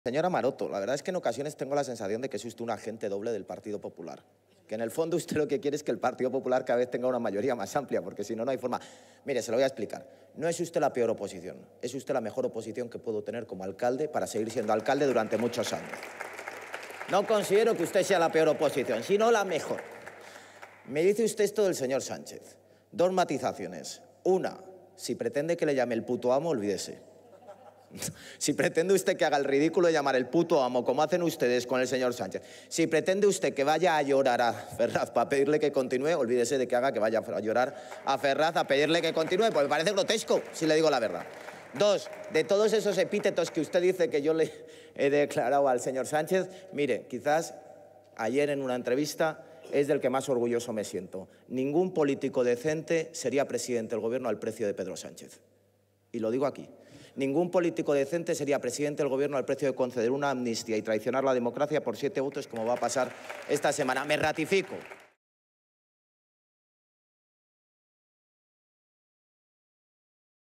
Almeida, a Maroto: «Si pretende que haga el ridículo de llamar 'puto amo' a Sánchez, olvídese»
El alcalde de Madrid, José Luis Martínez-Almeida, ha dado este martes durante el Pleno del Ayuntamiento de Madrid un repaso a la portavoz socialista, Reyes Maroto, y al secretario general de su formación, el presidente del Gobierno, Pedro Sánchez. «Si pretende que haga el ridículo de llamar 'el puto amo' a Sánchez, olvídese», le ha espetado desde la tribuna de oradores antes de asegurar que la ex ministra de Industria, Comercio y Turismo es «la mejor oposición» que podía tener para «seguir siendo alcalde de Madrid».